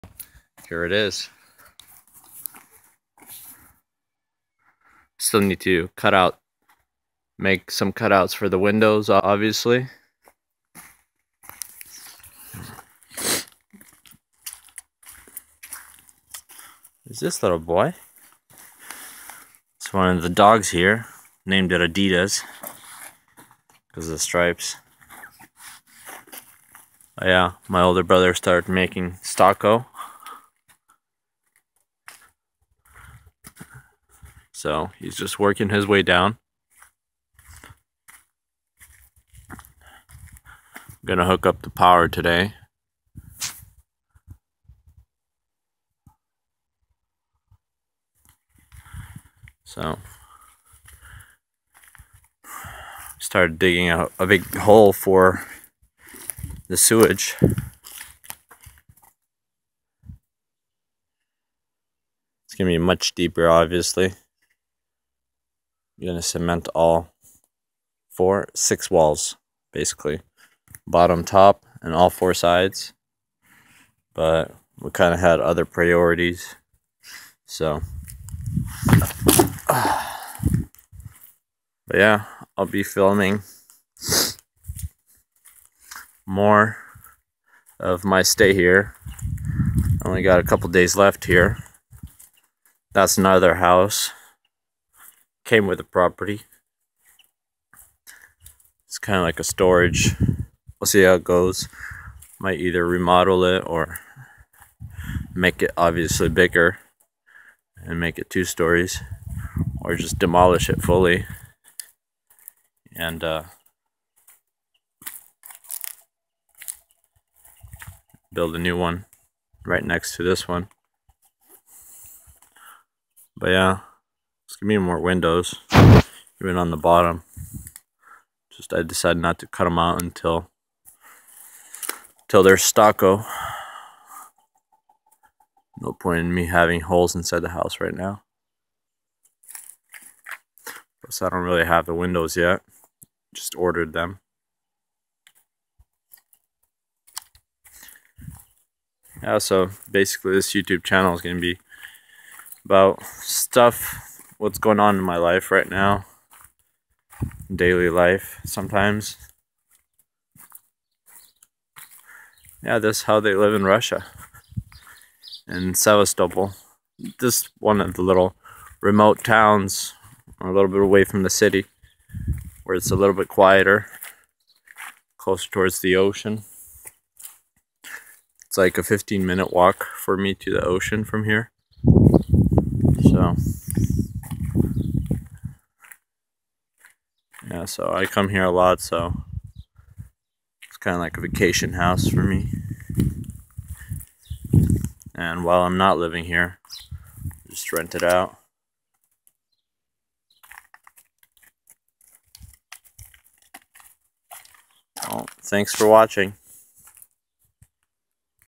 Building a vacation house in Sevastopol Russia
Im from California, United States and am trying to build a vacation house in Sevastopol Russia.